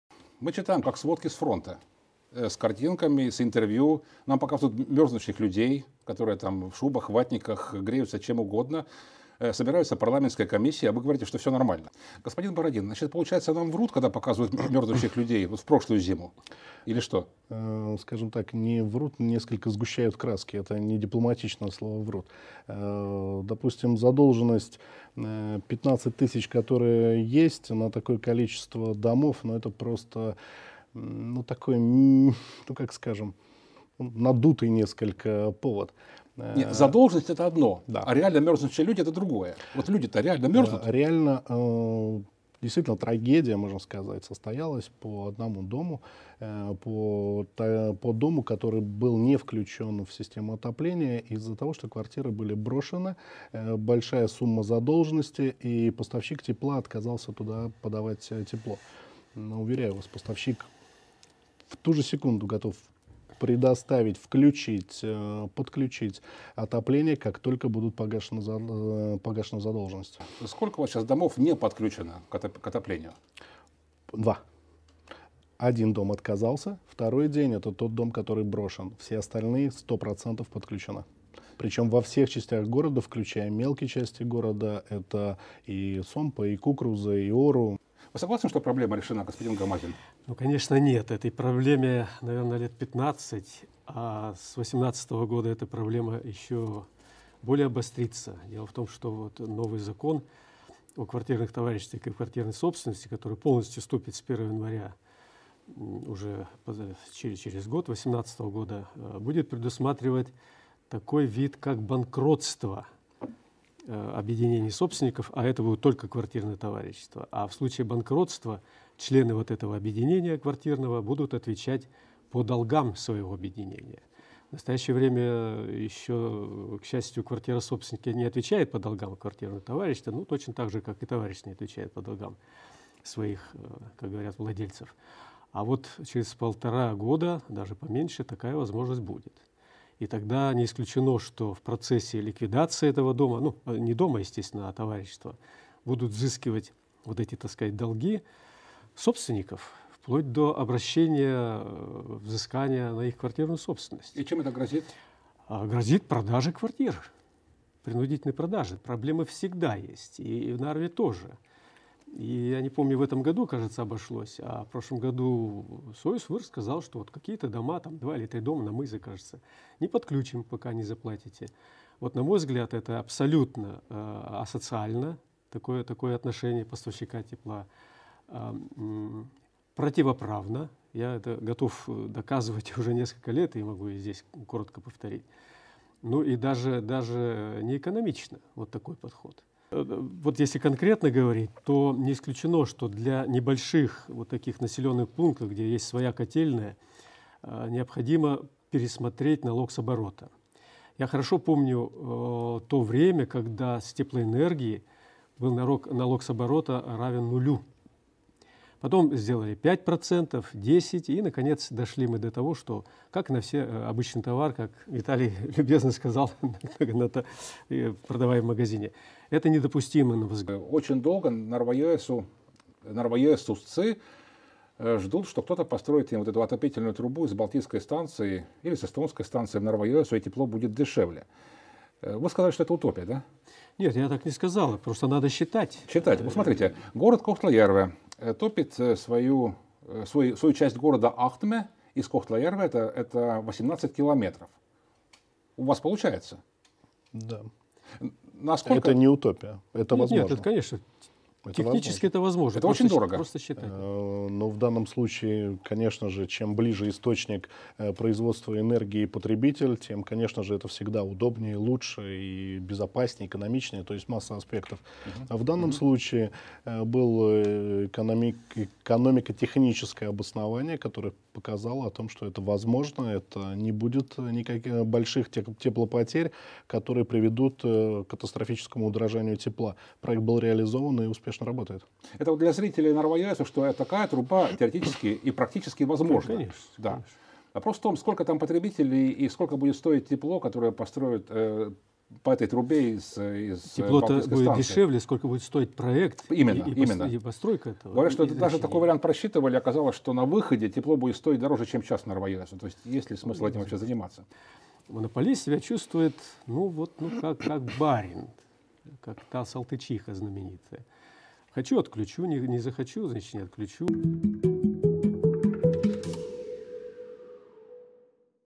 Грозит ли нам принудительная продажа квартир за коммунальные долги?
В Кохтла-Ярве сейчас без отопления два дома. Жильцы первого сами отказались от трубы и перешли на "альтернативные источники тепла", а второй, как говорит вице-мэр города Виталий Бородин, люди давно покинули. Кажется, что замерзать этой зимой никто не будет, но руководитель Нарвского объединения квартирных товариществ Александр Гамазин напоминает о долгах, неподъемных ценах на тепло для пенсионеров Ида-Вирумаа и новом законе о квартирных товариществах, который вступает в силу с 1 января 2018 года. Гамазин уверен, что дело пахнет керосином – принудительной продажей жилья.

Документ предусматривает такую процедуру банкротства КТ, что долги можно будет взыскивать с собственников жилья, рассказывает гость. В то же время Гамазин считает асоциальным, противоправным и неэкономичным отключение неплательщиков от трубы.

Также есть предложение пересмотреть налог с оборота для котелен в небольших населенных пунктах, ведь когда-то такой таксы вообще не было.

Что же касается проекта прокладки теплотрассы из Нарвы в Нарва-Йыэсуу, то технически такой проект возможен, но есть опасения, что в таком случае тепло будет стоить еще дороже, чем сейчас.